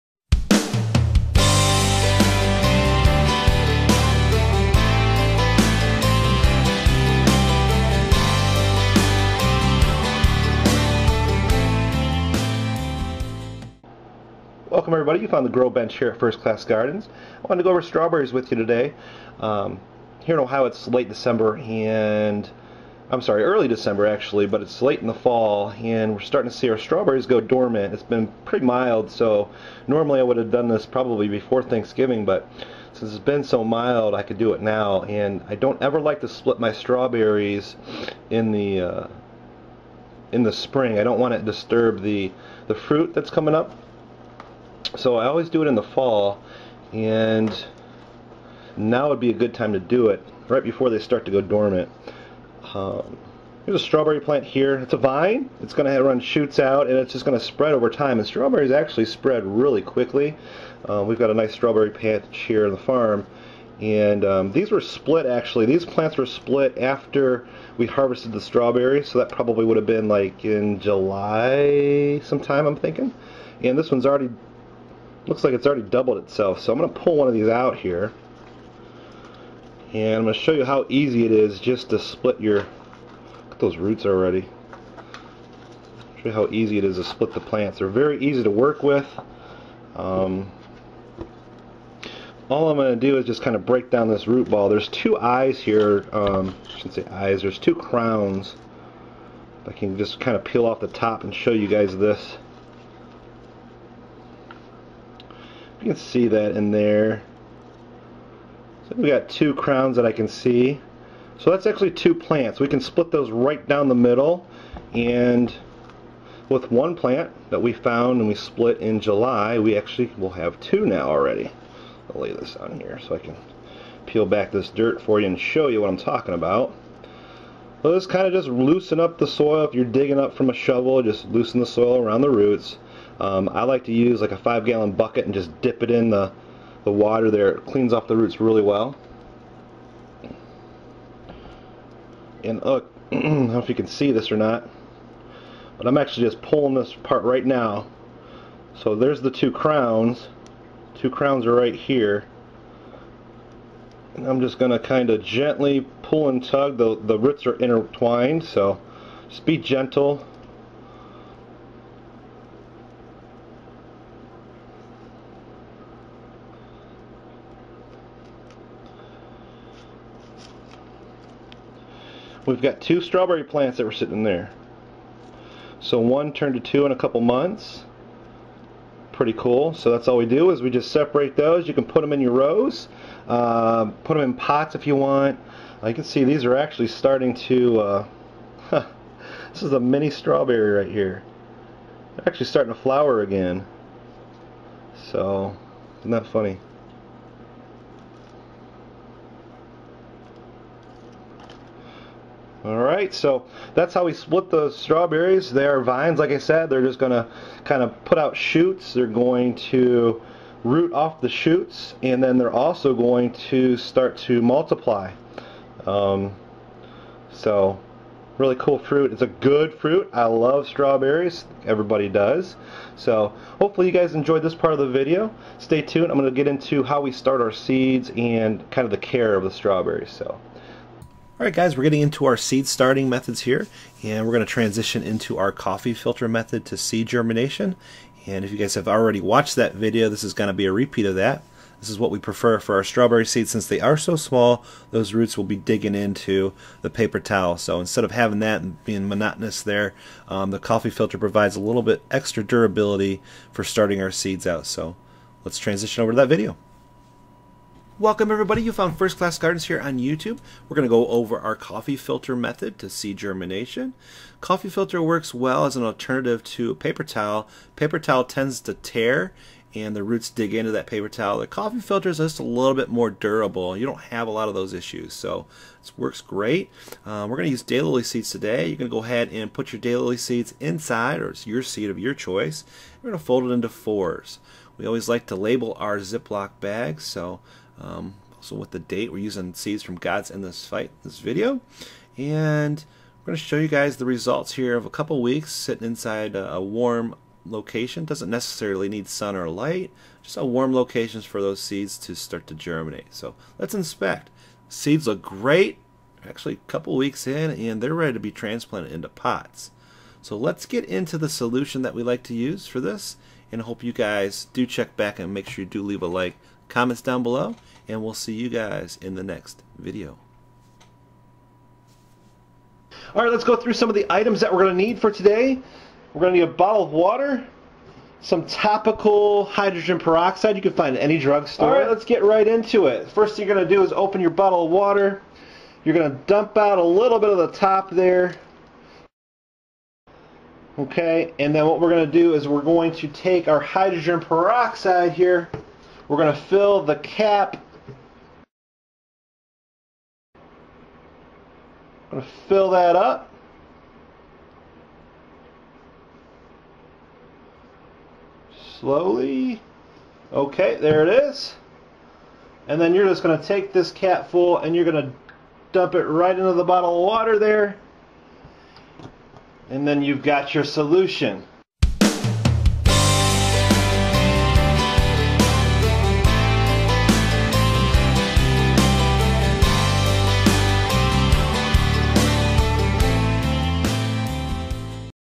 How To Grow Strawberries from Seed
How To Grow Strawberries from Seed growing strawberry plants in containers gardening organic fruit and Dividing Growing Strawberry Fruit.  Start Seeds Indoors at home.

Strawberries are the most popular small fruit grown in home gardens for good reason. Few things bring back summer memories like sinking teeth into a fresh and juicy strawberry. Why not have these sweet berries growing in your own garden? They are able to be grown in all the temperate regions of the world!

Strawberries are relatively easy to grow, and they can fit within small space constraints or fill huge garden plots. Growing strawberries doesn’t require any specialized equipment. And, they can even be grown in a container or pot on a deck, porch, patio, or balcony.

One of the biggest benefits of growing strawberries is their perennial nature. You can reap the rewards of your labor for several years after initially planting strawberries with minimal effort after the initial planting year. With proper care, it is not uncommon for each strawberry plant to produce a full quart of strawberries. Approximately twenty-five strawberry plants should adequately supply a normal family with delicious strawberries. how to grow strawberries in containers how to grow strawberries in pots gardening organic